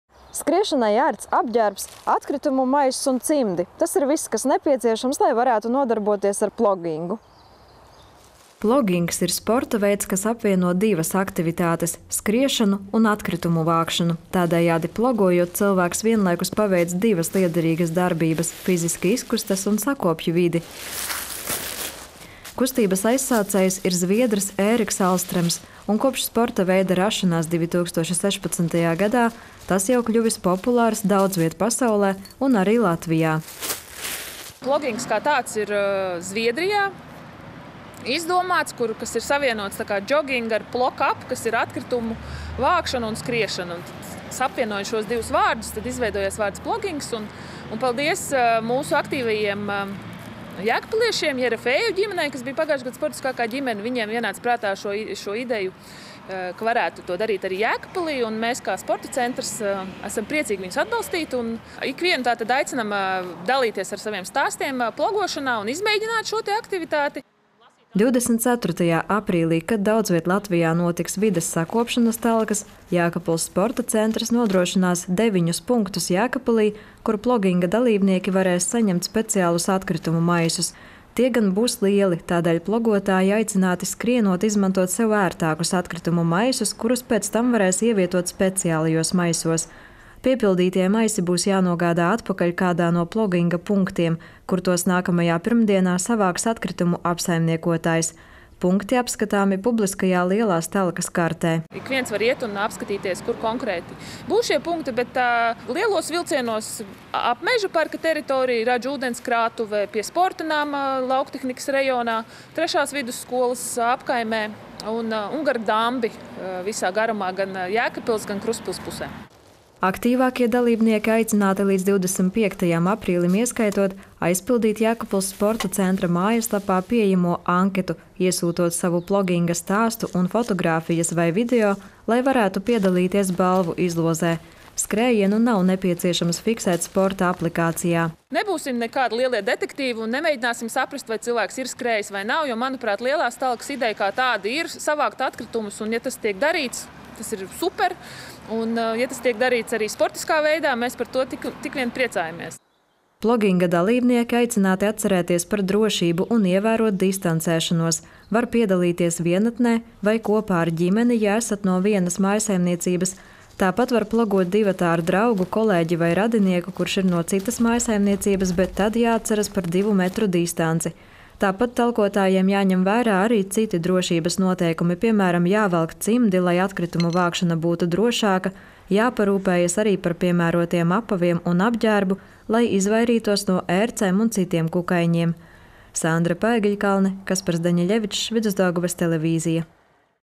Jēkabpils Sporta centrs aicina izmēģināt videi draudzīgu sporta veidu – plogingu
Jau šajā sestdienā ikviens aicināts pielikt savu roku, lai Latvijas vide kļūtu tīrāka un sakoptāka. Ik gadu tiek savākti daudzi maisi ar atkritumiem, iestādīti daudzi koki, krūmi un puķes, tāpat veikti citi apkārtnes labiekārtošanas darbi. Bet pirms vairākiem gadiem pasaulē parādījusies vēl kāda aktivitāte, kas saistās ar vides sakopšanu – plogings. Pamazām tas kļūst populārs arī Latvijā, bet pirmo reizi to piedāvā izmēģināt arī Jēkabpilī.

Lai varētu nodarboties ar plogingu, nepieciešams skriešanai ērts apģērbs, atkritumu maiss un cimdi. Plogings ir sporta veids, kas apvieno divas aktivitātes – skriešanu un atkritumu vākšanu, tādējādi plogojot cilvēks vienlaikus paveic divas lietderīgas darbības – fiziski izkustas un sakopj vidi. Kustības aizsācējs ir zviedrs Ēriks Alstrems, un kopš sporta veida rašanās 2016.gadā tas jau kļuvis populārs daudzviet pasaulē un arī Latvijā.

“Vārds “plogings” radies, apvienojot divus vārdus - “jogging” un “plocka upp”, kas nozīmē – skriešana un atkritumu vākšana,” stāsta Jēkabpils sporta centra sporta organizatore Dace Zariņa. “Paldies mūsu aktīvajiem jēkabpiliešiem – Jerofejevu ģimenei, kas bija pagājušogad gada sportiskākā ģimene. Viņiem ienāca prātā, ka varētu to darīt arī Jēkabpilī, un mēs kā sporta centrs esam priecīgi viņus atbalstīt un ikvienu aicinām dalīties ar saviem stāstiem plogošanā un izmēģināt šo aktivitāti.”

24.aprīlī, kad daudzviet Latvijā notiks vides sakopšanas talkas, Jēkabpils sporta centrs nodrošinās deviņus punktus Jēkabpilī, kur ploginga dalībnieki varēs saņemt speciālus atkritumu maisus. Tie gan būs lieli, tādēļ plogotāji aicināti skrienot izmantot sev ērtākus atkritumu maisus, kurus pēc tam varēs ievietot speciālajos maisos. Piepildītie maisi būs jānogādā atpakaļ kādā no ploginga punktiem, kur tos nākamajā pirmdienā savāks atkritumu apsaimniekotājs. Punkti apskatāmi publiskajā Lielās Talkas kartē. “Ikviens var apskatīt kartē, kur konkrēti šie punkti būs, bet tie būs ap Mežaparka un Radžu ūdenskrātuves teritoriju, pie Sporta nama Lauktehnikas rajonā, Jēkabpils 3.vidusskolas apkaimē un gar dambi gan Jēkabpils, gan Krustpils pusē,” stāsta Dace Zariņa.

Aktīvākie dalībnieki aicināti līdz 25.aprīlim ieskaitot aizpildīt Jēkabpils sporta centra mājaslapā pieejamo anketu, iesūtot savu ploginga stāstu un fotogrāfijas vai video, lai varētu piedalīties balvu izlozē. Skrējienu nav nepieciešams fiksēt sporta aplikācijā – uzsver Dace Zariņa: “Nebūsim nekādi lielie detektīvi un nemēģināsim saprast, vai cilvēks ir skrējis, vai nav. Lielās Talkas ideja ir savākt atkritumus. Ja tas tiek darīts, ir super, ja tas tiek darīts sportiskā veidā, mēs par to tikai priecājamies.”

Ploginga dalībnieki aicināti atcerēties par drošību un ievērot distancēšanos. Var piedalīties vienatnē vai kopā ar ģimeni, ja esat no vienas mājsaimniecības, tāpat var plogot divatā ar draugu, kolēģi vai radinieku, kurš ir no citas mājsaimniecības, bet tad jāatceras par divu metru distanci. Tāpat talkotājiem jāņem vērā arī citi drošības noteikumi, piemēram, jāvelk cimdi, lai atkritumu vākšana būtu drošāka, jāparūpējas arī par piemērotiem apaviem un apģērbu, lai izvairītos no ērcēm un citiem kukaiņiem.

Sandra Paegļkalne